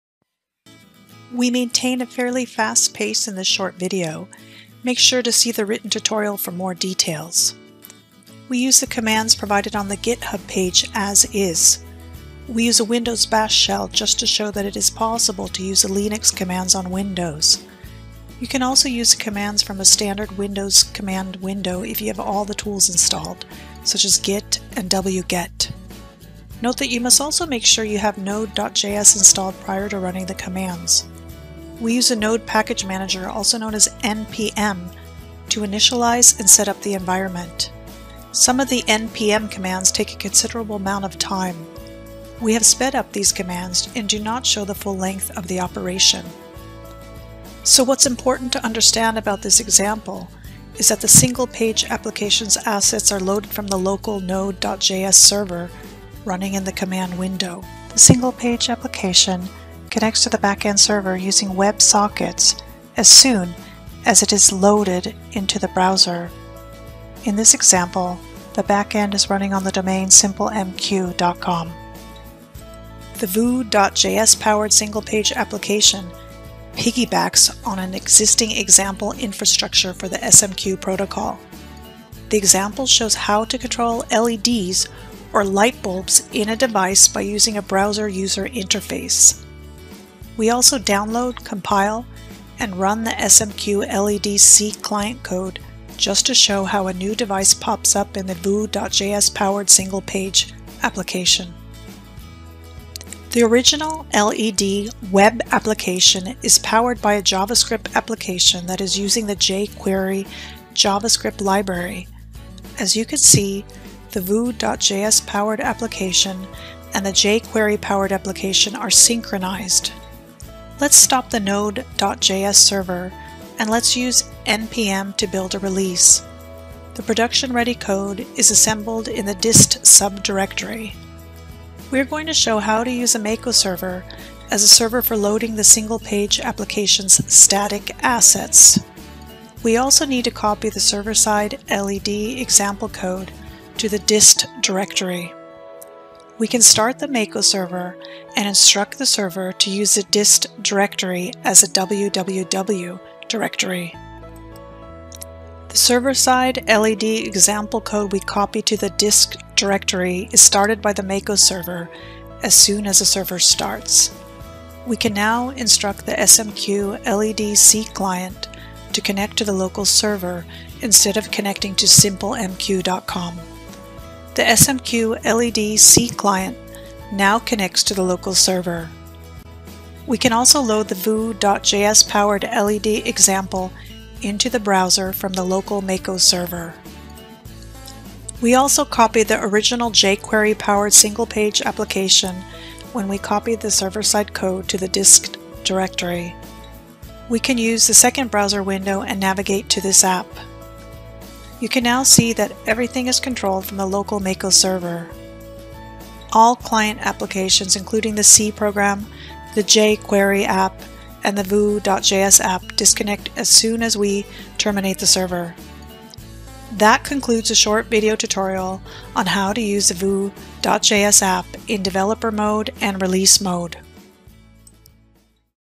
Using Vue.js with the Mako Server
We maintain a fairly fast pace in this short video. Make sure to see the written tutorial for more details.

We use the commands provided on the GitHub page "as is". We use  a Windows bash shell just to show that it is possible to use the Linux commands on Windows. You can also use the commands from a standard Windows command window if you have all the tools installed, such as 'git' and 'wget'.

Note that you must also make sure you have Node.js installed prior to running the commands.

We use the Node Package Manager aka npm to initialize and set up the environment.

Some of the npm commands take a considerable amount of time. We have sped up these commands and do not show the full length of the operation.

So what's important to understand about this example is that the Single Page Application's assets are loaded from the local Node.js server running in the command window. The Single Page Application connects to the backend server using WebSockets as soon as it is loaded into the browser. In this example, the backend is running on the domain simplemq.com.

The Vue.js powered Single Page Application piggy backs on an existing example infrastructure for the SMQ protocol. The example shows how to control LEDs or light-bulbs in a device by using a browser user interface.

We also download, compile, and run the SMQ LED C client code just to show how a new device pops up in the Vue.js powered Single Page application.

The original LED web application is powered by a JavaScript application that is using the JQuery JavaScript library. As you can see, the Vue.js powered application and the JQuery powered application are synchronized.

Let's stop the Node.js server and lets use npm to build a release. The production ready code is assembled in the 'dist' sub directory.

We are going to show how to use the mako server as the server for loading the Single Page Application's static assets. We also need to copy the server side LED example code to the 'dist' directory.

We can start the mako server and instruct the server to use the 'dist' directory as the 'www' directory.

The server side LED example code we copied to the 'dist' directory is started by the Mako Server as soon as the server starts. We can now instruct the SMQ LED C client to connect to the local server instead of connecting to simplemq.com.

The SMQ LED C client now connects to the local server.

We can also load the Vue.js powered LED example into the browser from the local Mako Server.

We also copied the original JQuery powered Single Page application when we copied the server side code to the 'dist' directory. We can use the second browser window and navigate to this app.

You can now see that everything is controlled from the local Mako server.

All client applications, including the C program, the JQuery app, and the Vue.js app disconnect as soon as we terminate the server.

That concludes the short video tutorial on how to use the Vue.js app in developer mode and release mode.